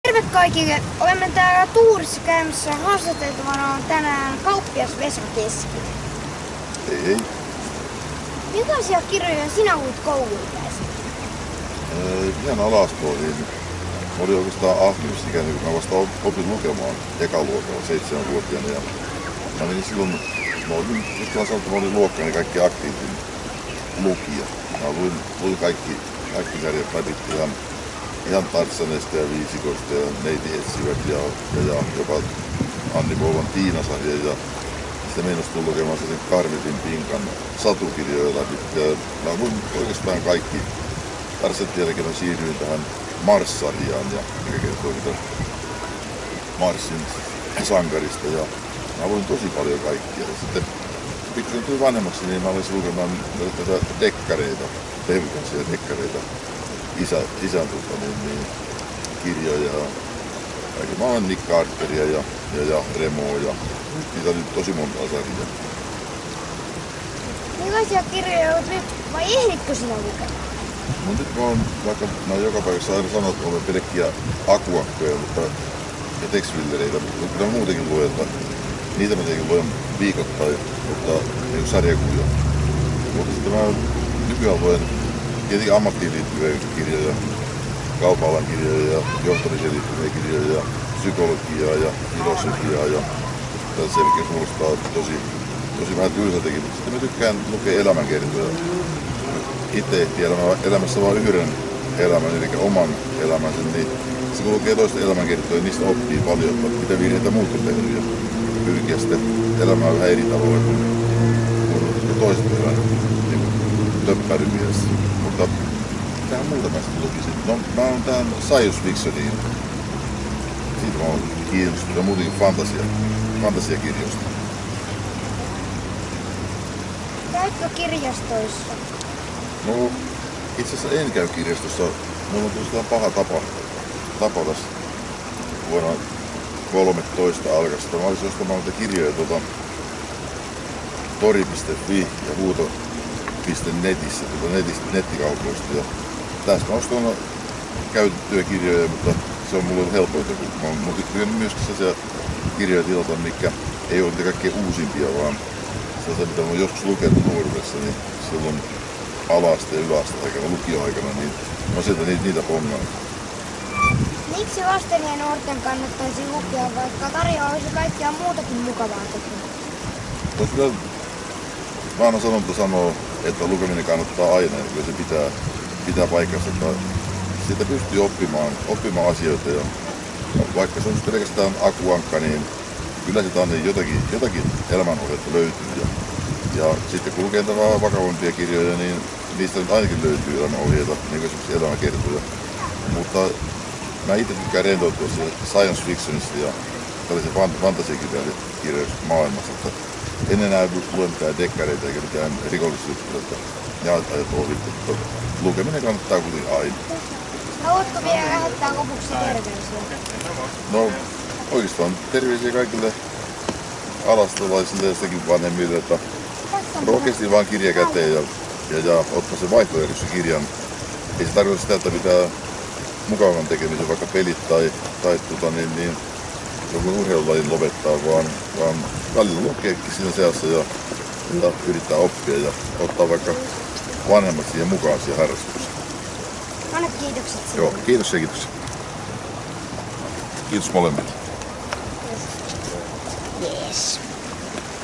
Kauppias Vesa Keskisen haastattelu lukemisesta
Kauppias Vesa Keskisen haastattelu lukemisesta, Tuuri Kyläkauppa Veljekset Keskinen heinäkuu 2019.